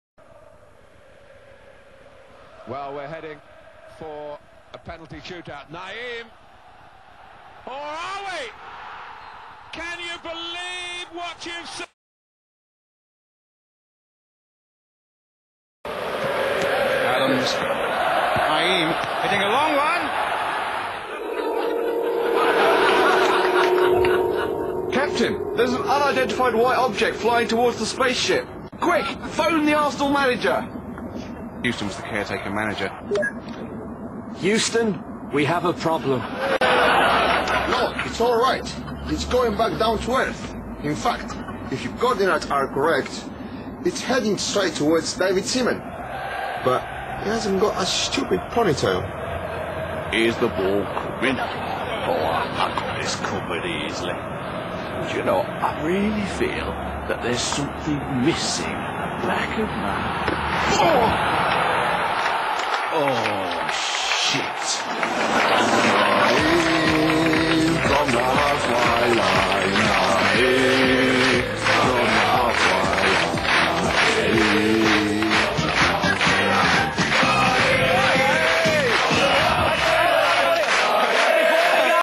Nayim From The Halfway Line!
Nayim scoring from the halfway line for Real Zaragoza against Arsenal in the Cup Winner's Cup Final!